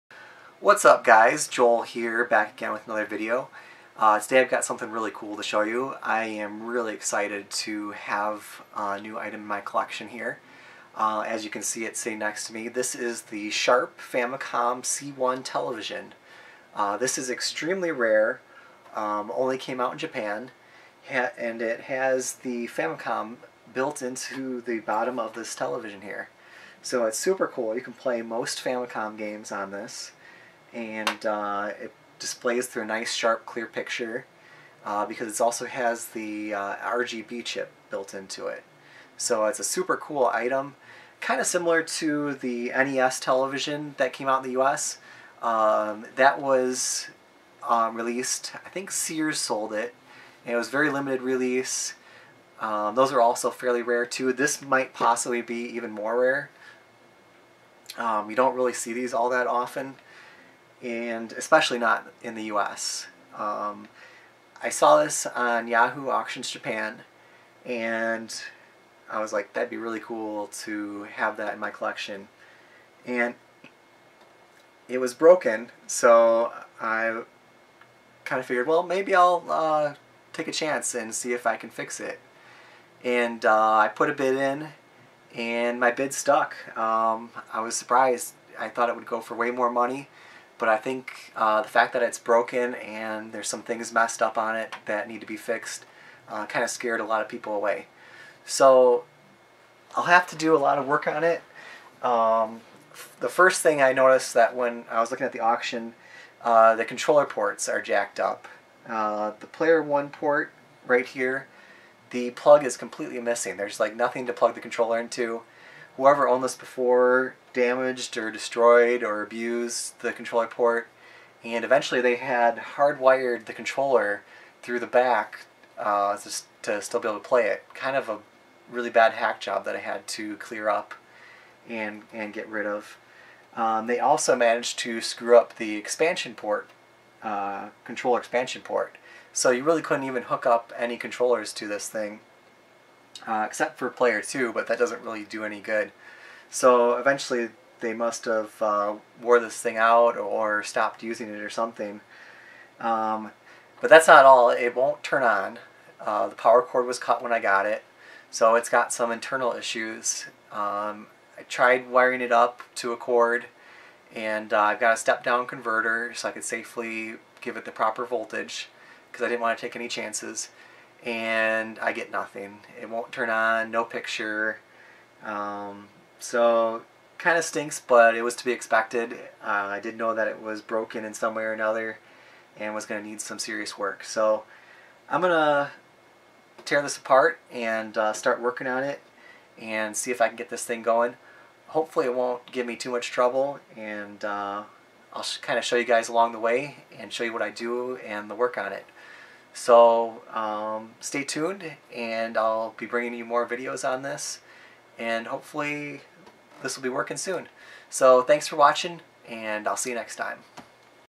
Sharp C1 Famicom TV Repair Log Pt 1 (14C-C1R)
Can't believe I found one of these!!  Now it's time to get it working!!  Very similar to the NES TV (19SV111 and 19SC111) that was released in the US, but it has RGB!!!